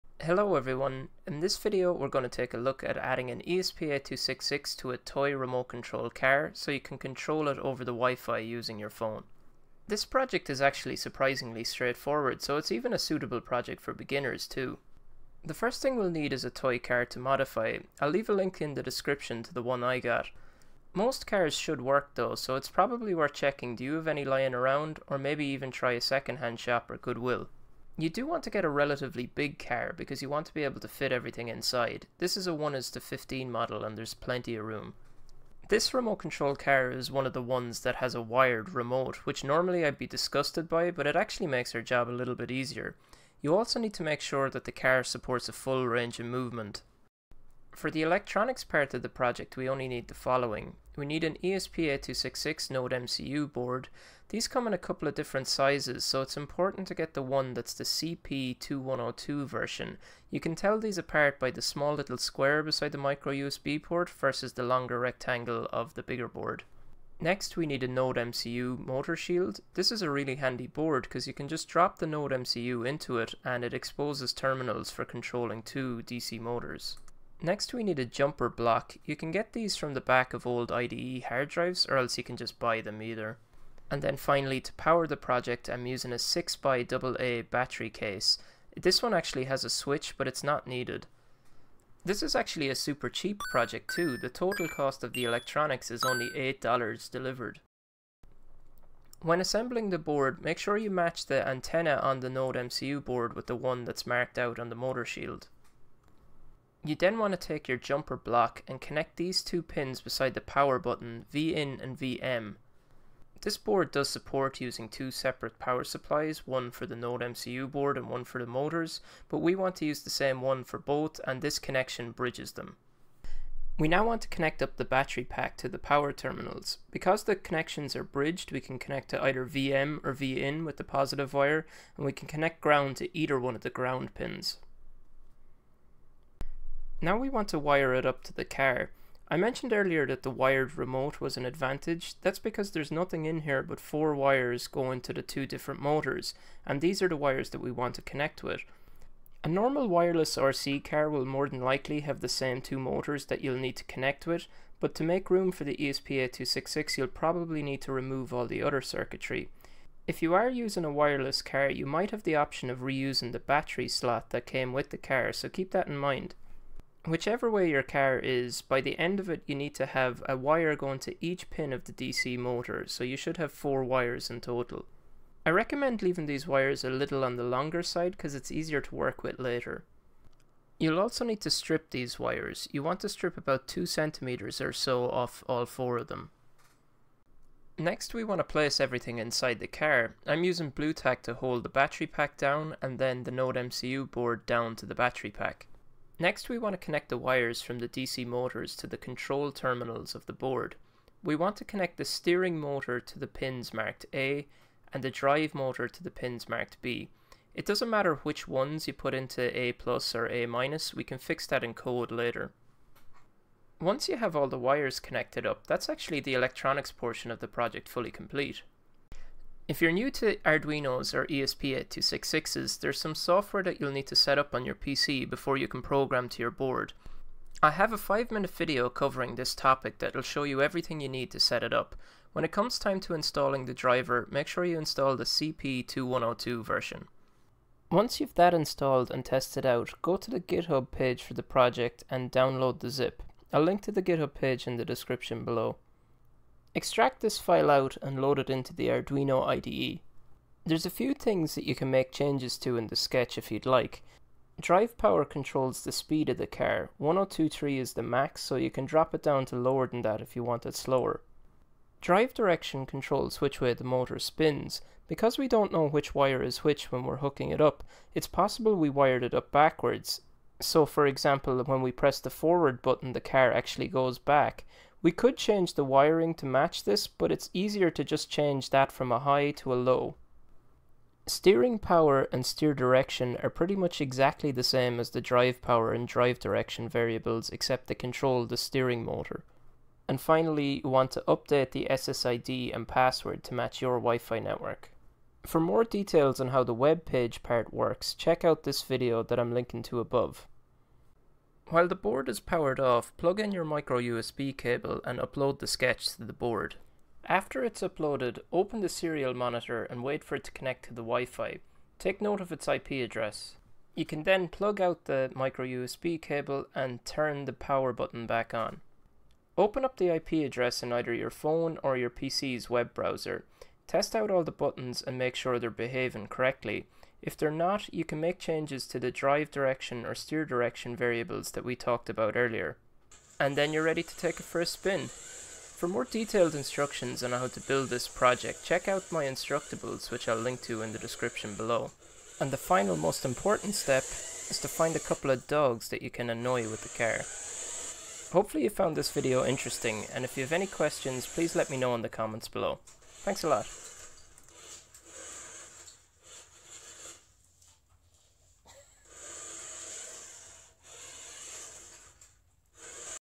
Adding WiFi controls to a RC car using Arduino
In this video I'm going to show you how to add WiFi controls to a toy remote control car using an ESP8266.

This is actually a really simple project, so it would actually make a good project for someone pretty new to Arduino. Even the way I show it in the video I did not even need to solder for it.

NodeMCU ESP8266 board*

NodeMCU Motor Shield*

6 X AA battery case*

Jumper Block (Pack of 60)*

Logitech C920 HD 1080p Pro webcam